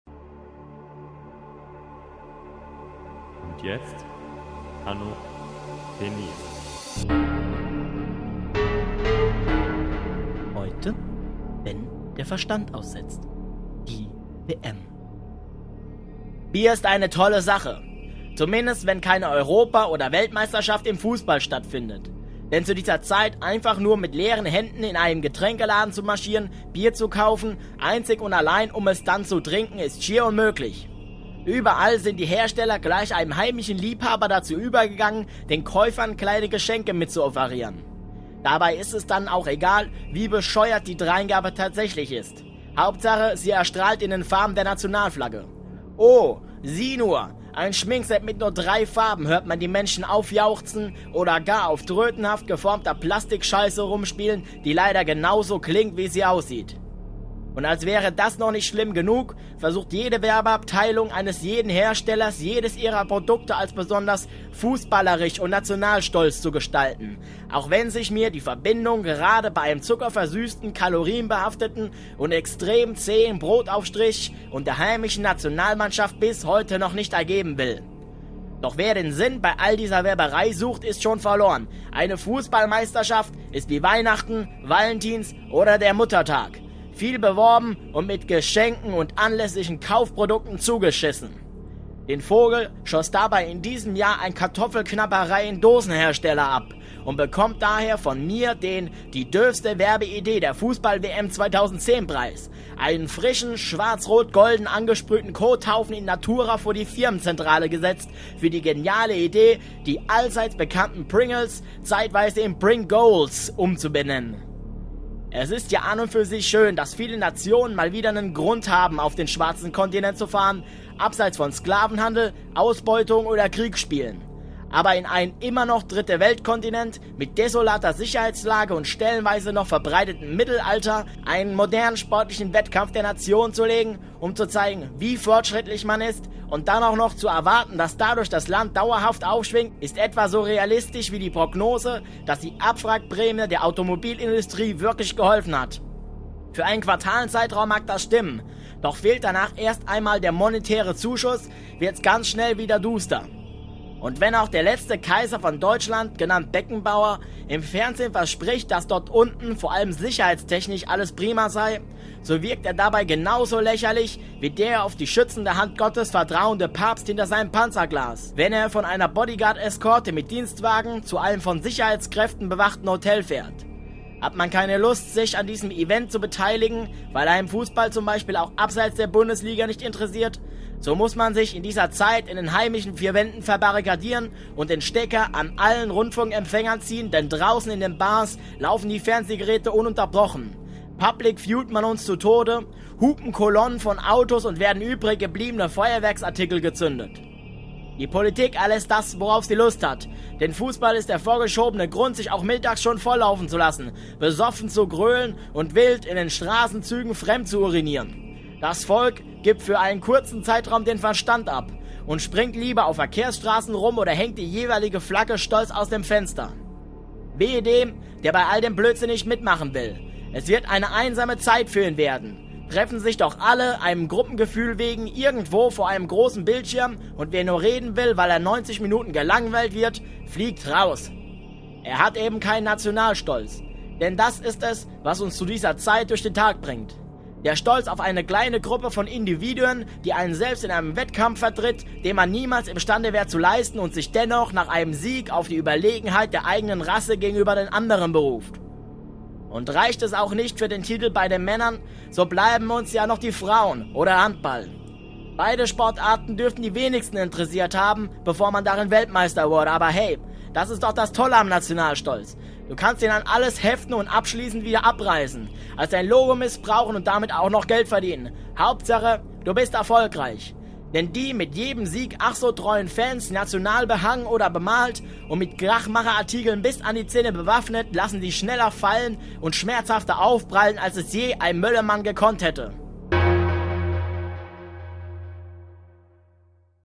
Hanno Peniis - Folge 15 - Wenn der Verstand aussetzt (Die WM)
Ole-Ola-Gejubel zwischen Bier und Geschlechtsverkehr. Bin ich da stolz Deutscher zu sein?